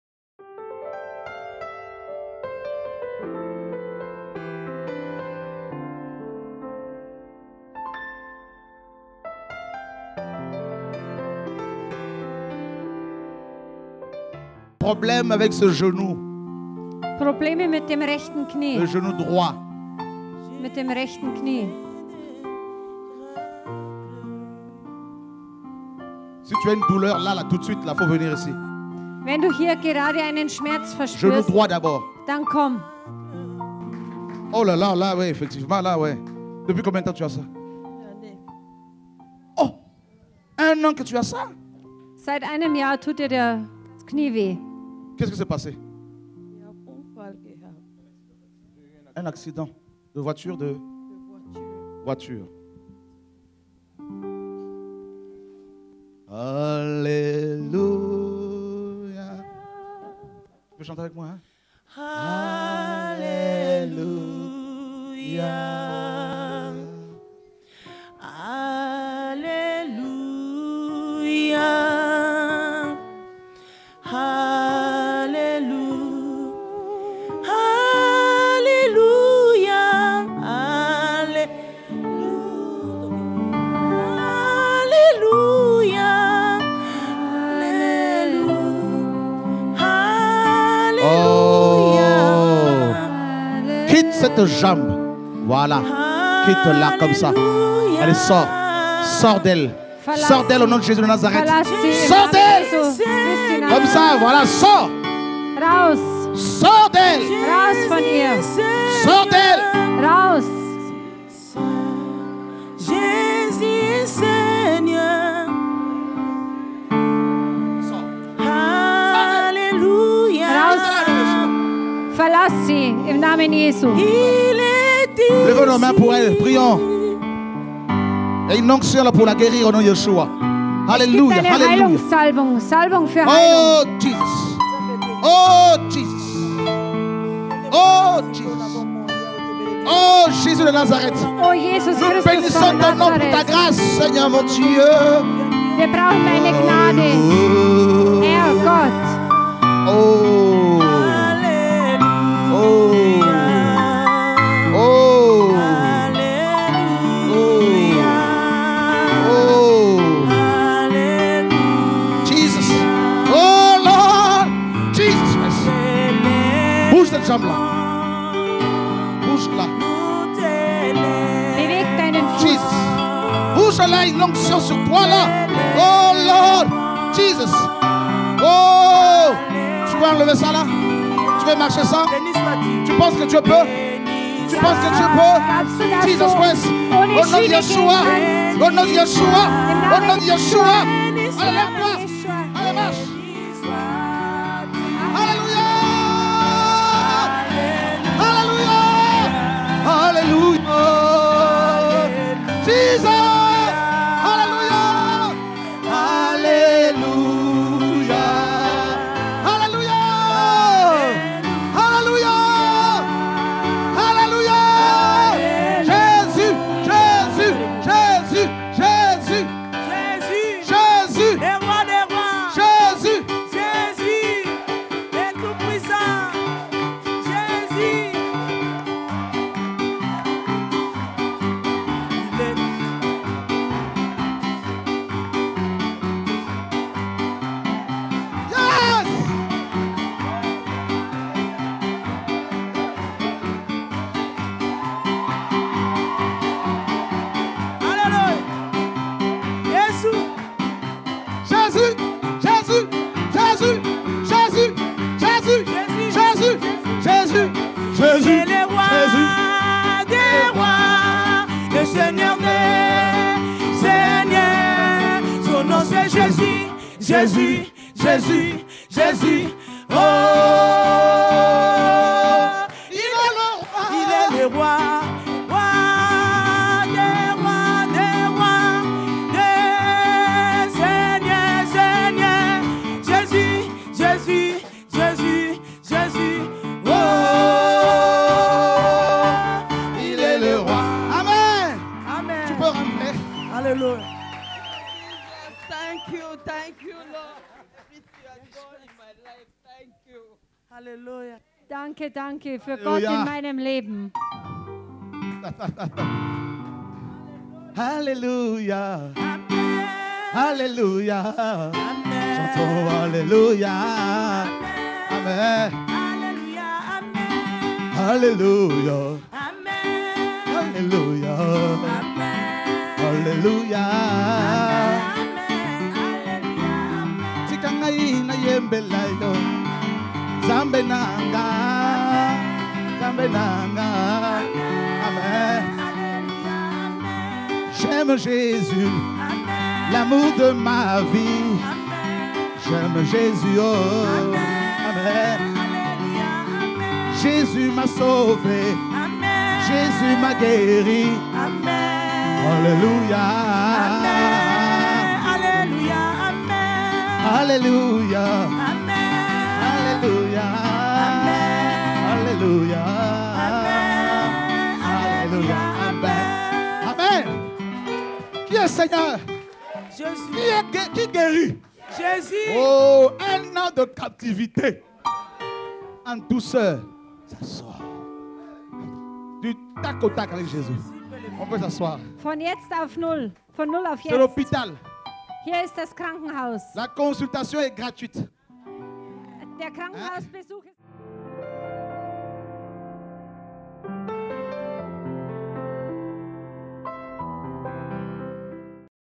Jésus guérit une femme après un accident - Stuttgart (fr/de) (Shora Kuetu - 19/10/14)
"Et voici les miracles qui accompagneront ceux qui auront cru : Ils chasseront les démons en mon Nom ; ils parleront de nouvelles langues ; ils saisiront les serpents avec la main, et s'ils boivent quelque breuvage mortel, il ne leur fera pas de mal ; ils imposeront les mains aux malades, et ils seront guéris."
(Marc 16:17-18)